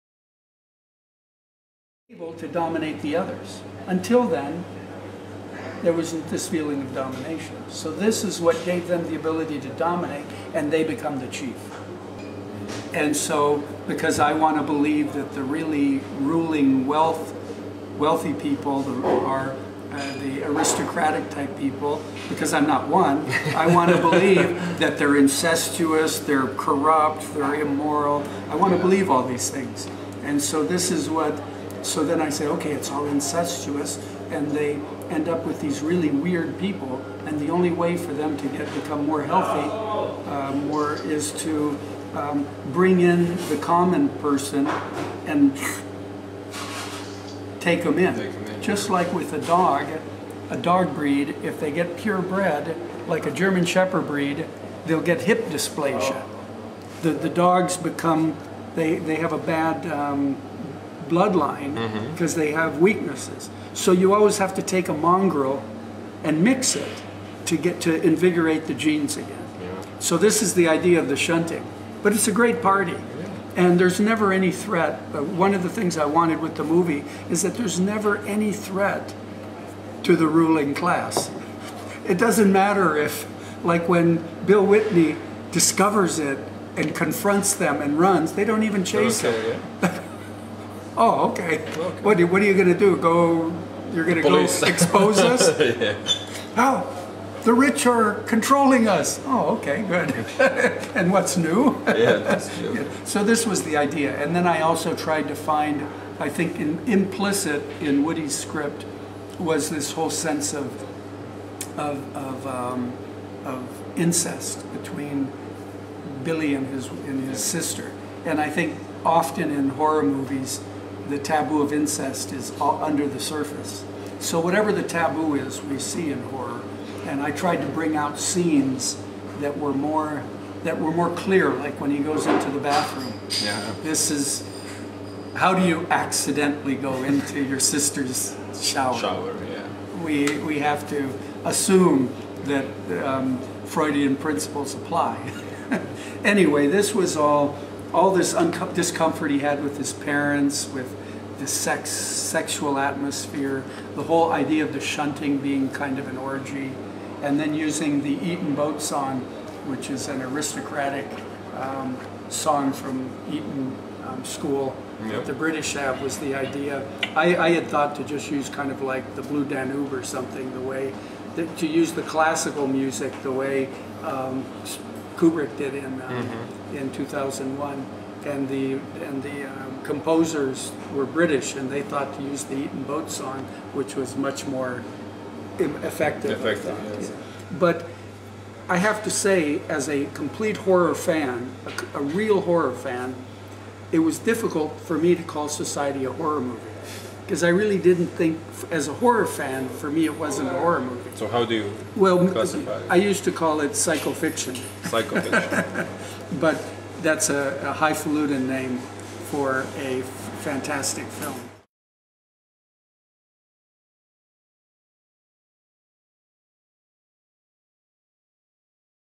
Brian Yuzna: Up close & personal at Grossmann 2008 - part 3/4
Did you know that Brian Yuzna has Slovenian heritage?
Did you know that Brian Yuzna was a hippie?
Did you know that Brian Yuzna had goats? Wanna know why?

Watch exclusive interview with Brian Yuzna and see how he get into the movie business, why he had goats and wasn't a shepperd, what he did as a hippie in his early years, how he get into L.A., what inspired him to make "Society", who's the real Bill Whitney from "Society", why "Re-Animator", what's the future of "Re-Animator", news about "House of Re-Animator" and hear more never before heard exclusive stuff from Brian himself.

*** Brian Yuzna: Up close & personal at Grossmann 2008 ***

* Part 3/4: continues about "Society"

Shot at Grossmann Film & Wine festival 2008
Ljutomer, Slovenia

Interview conducted and moderated by Velimir Grgic

Video by Arminio

Due to YouTube policy of 10 minute video duration, video is divided in 4 parts.

Total duration: approx. 32 minutes

Part 3: playing...

Note: interview is shot in a hotel lobby so some background noise is heard occasionally.